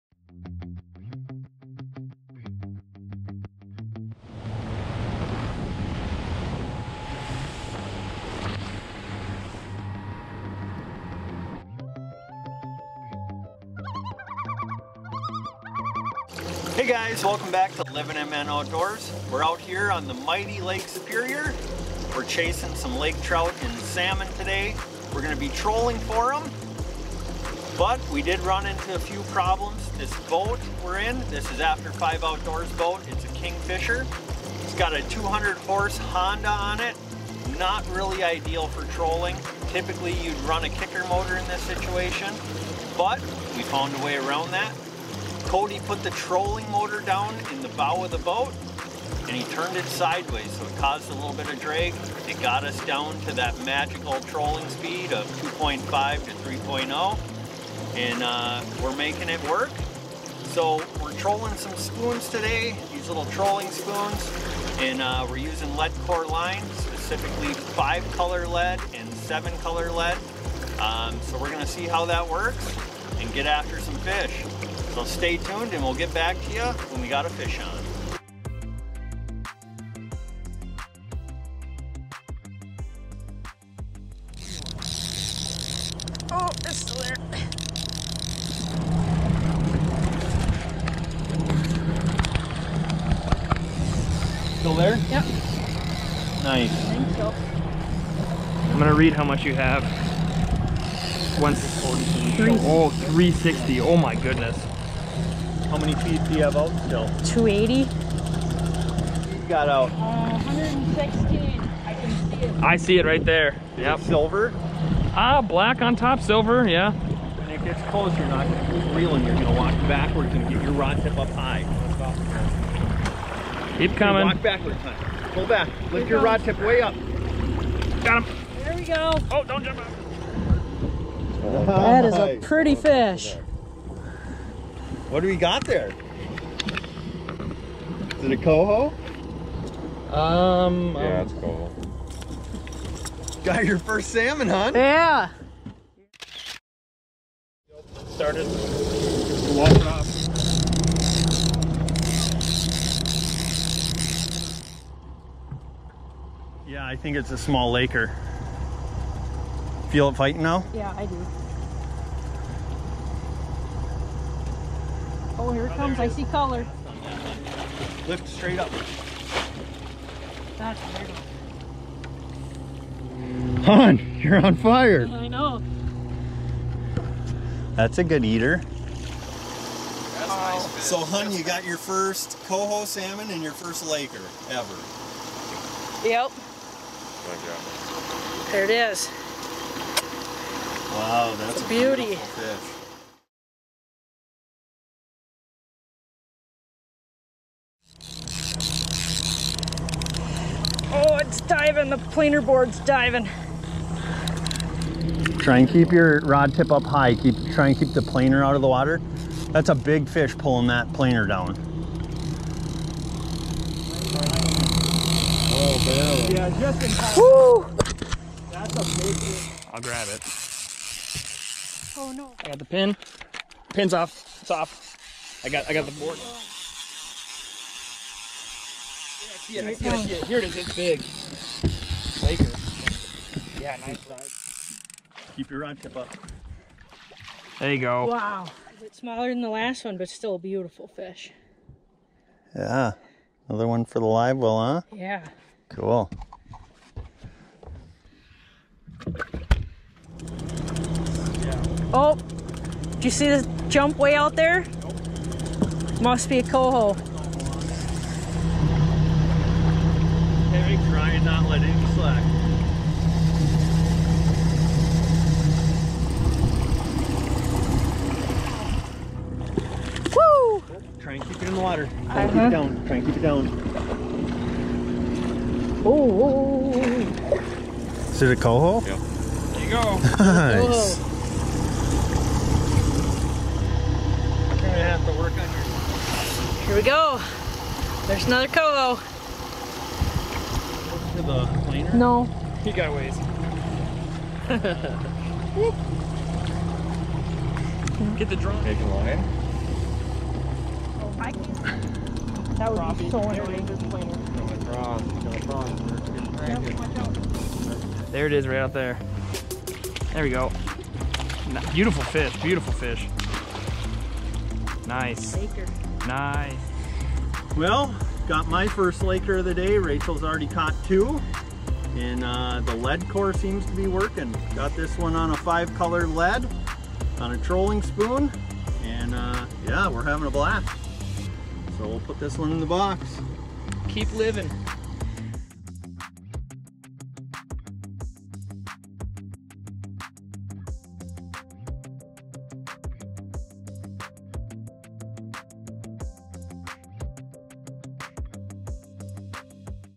Trolling for Trout and Salmon on Lake Superior 2021
We went trolling for salmon and trout on the north shore of Lake Superior. We went out with After 5 Outdoors and he put us on the fish! We trolled with lead core and planer boards and found ourselves to be quite successful! We hope you enjoy watching us catching some big Lake Superior trout and salmon.